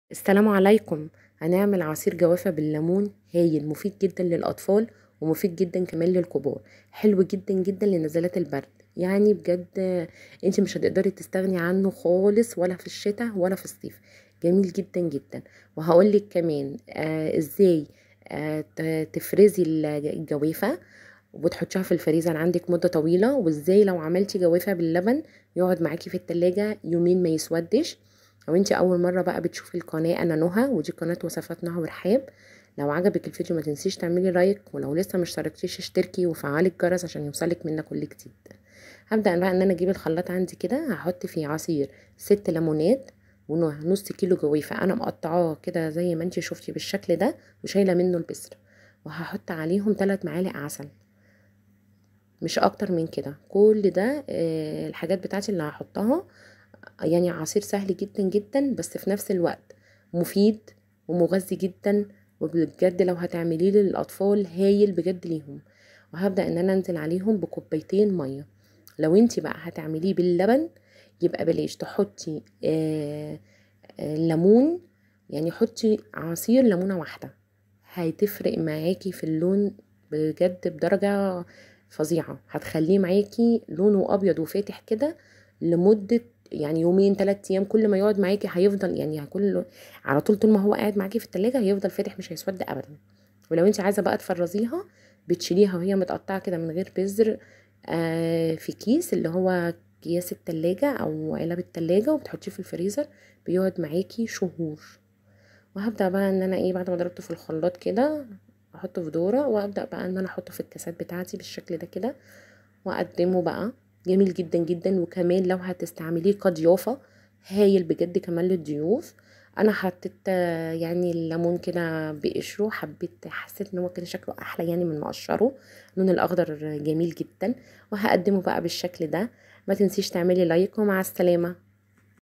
افضل عصير جوافة باليمون 🥤للكبار والأطفال(مفيد لاعراض البرد الحاده)
طريقة عمل عصير جوافة باليمون صحى جدا ومفيد للكبار والأطفال وكمان مفيد لنزلات البرد الحاده

المقادير

١/٢ كيلو جوافة منزوعة البزر
٣ ملاعق عسل نحل🍯
عصير ٦ ليمونة
٢كوب ماء